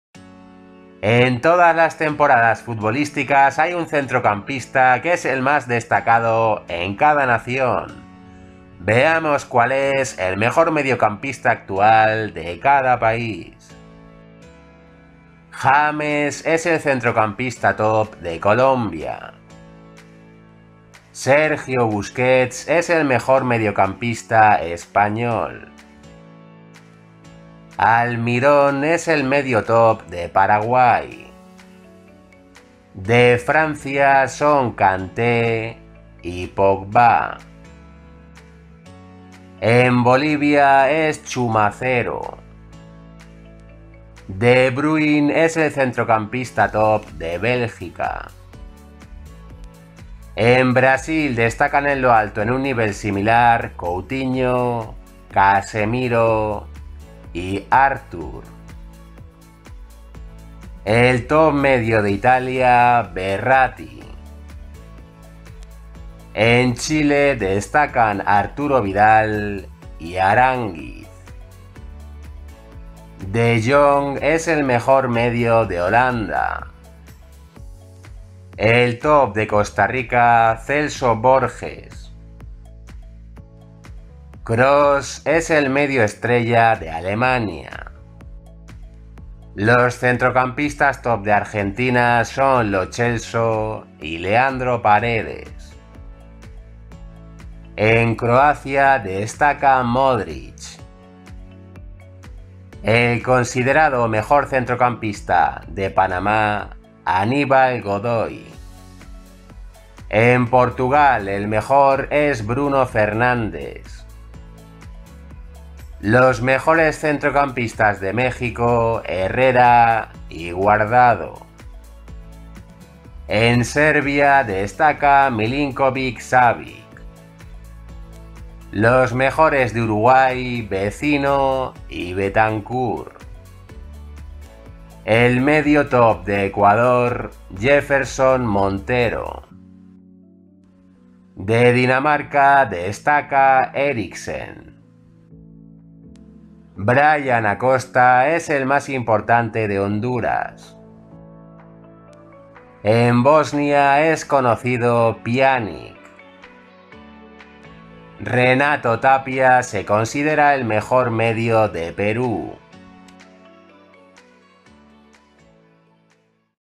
EL MEJOR MEDIO DE CADA PAÍS
El mejor centrocampista actual de cada país.

Like, comparte y elige el contenido.